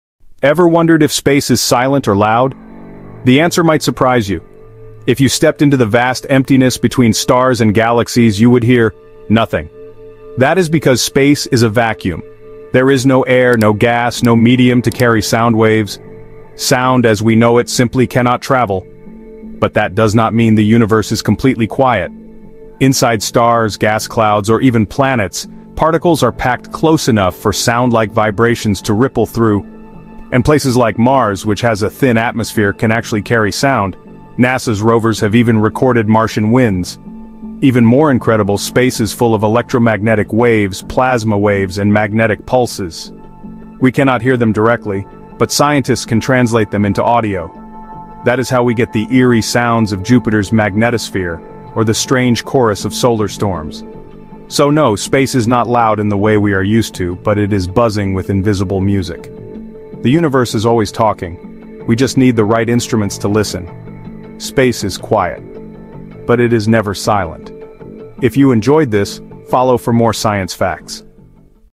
Is Space Silent or Buzzing?
Ever wondered if space is silent or loud? Prepare to be surprised! In this captivating video, we explore the intriguing nature of sound in the cosmos. Discover why the vast emptiness between stars is eerily quiet and how sound waves can't travel through a vacuum.

But wait—space isn't completely silent! Inside stars and gas clouds, sound-like vibrations ripple, and even Mars has been known to carry sound. Learn about the electromagnetic waves and plasma pulses that create a symphony of unseen sounds, from Jupiter’s magnetosphere to the eerie tones of solar storms.

Join us on this cosmic journey and uncover the true nature of the universe!